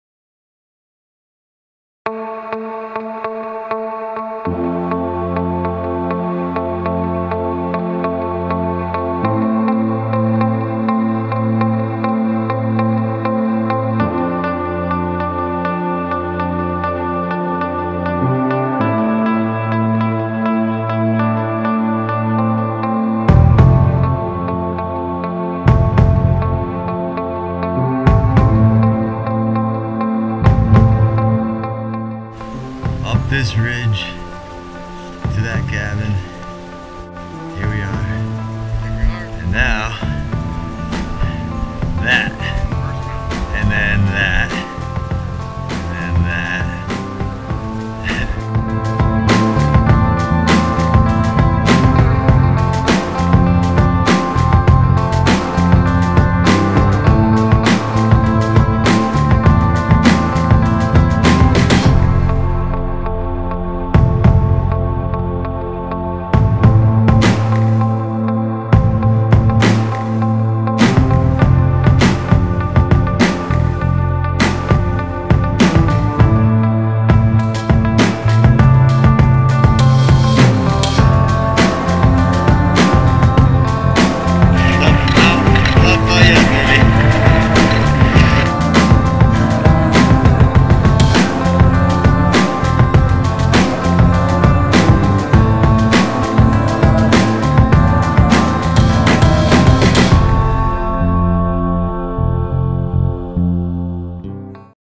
Mt Lafayette Loop Winter Climb
Mt Lafayette, Mt Lincoln and Little Haystack in 6 hours. Perfect weather beautiful climb in the White Mountains.